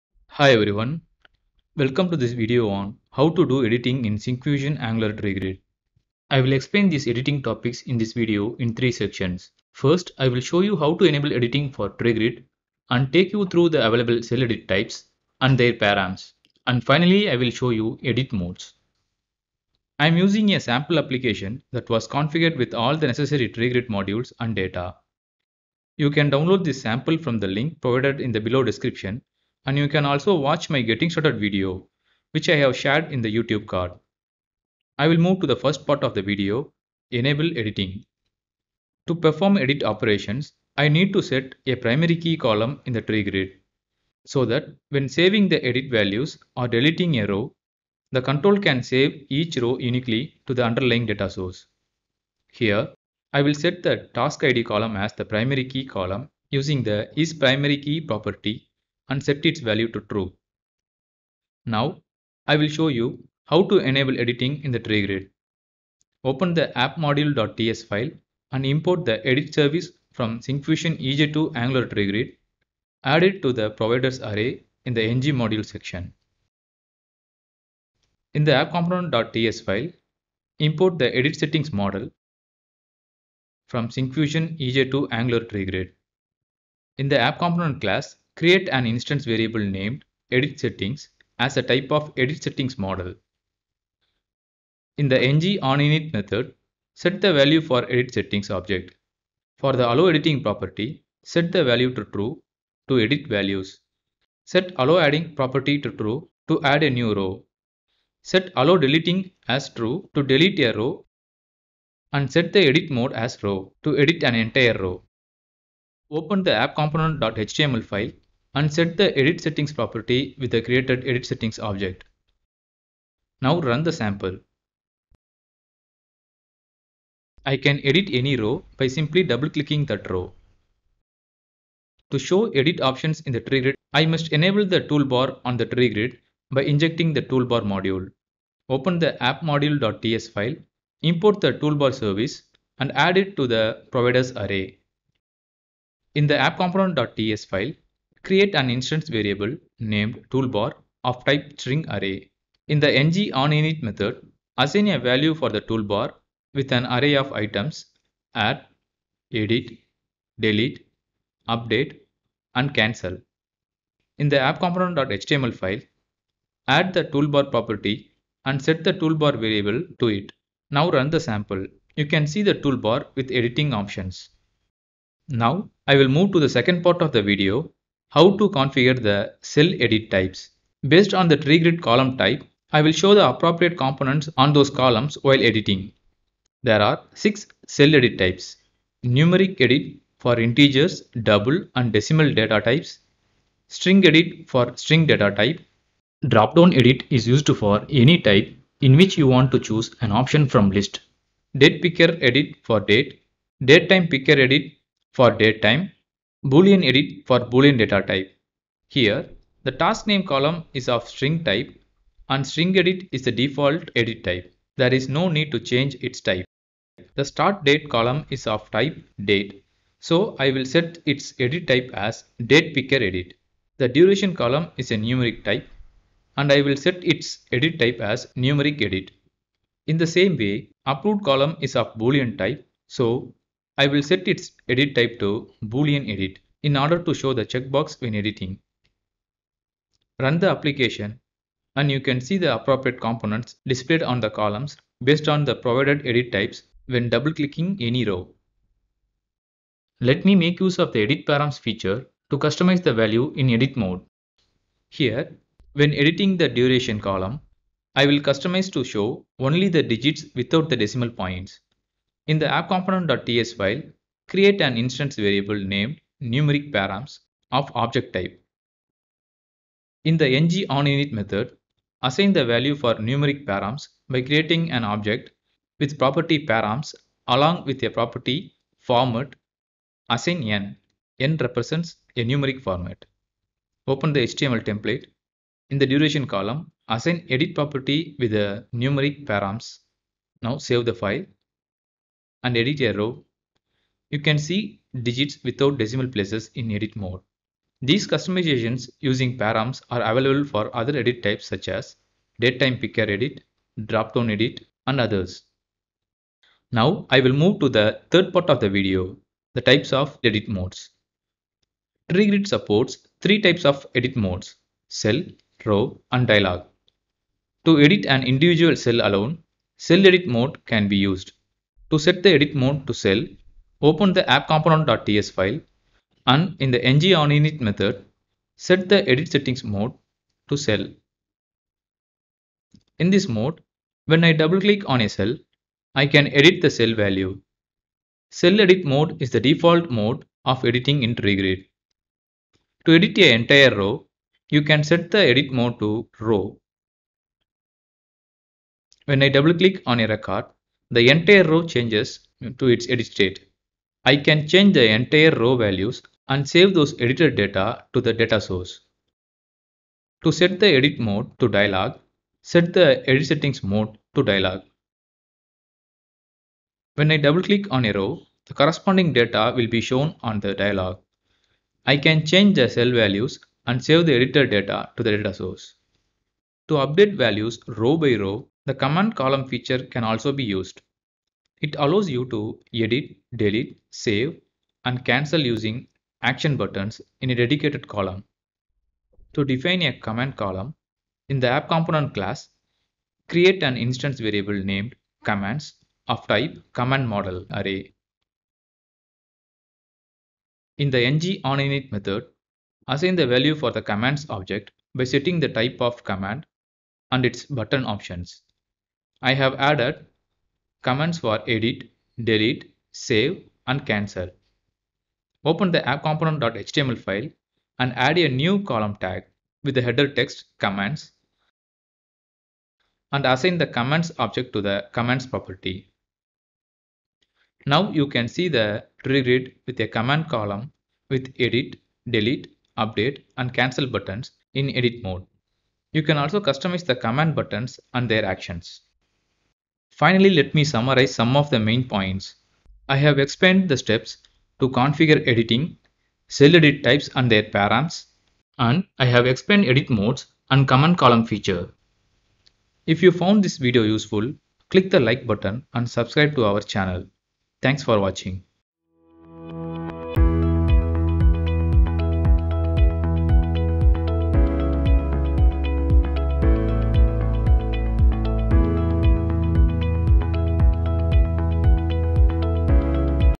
Different Editing Modes in Angular Tree Grid
Learn how you can edit cells in Syncfusion Angular Tree Grid.
I’ll explain the Tree Grid editing features like edit types, edit modes, and Command column.

[00:34] Enable editing
[02:55] Edit types
[04:09] Format values using edit params
[05:06] Edit modes
[06:16] Command column editing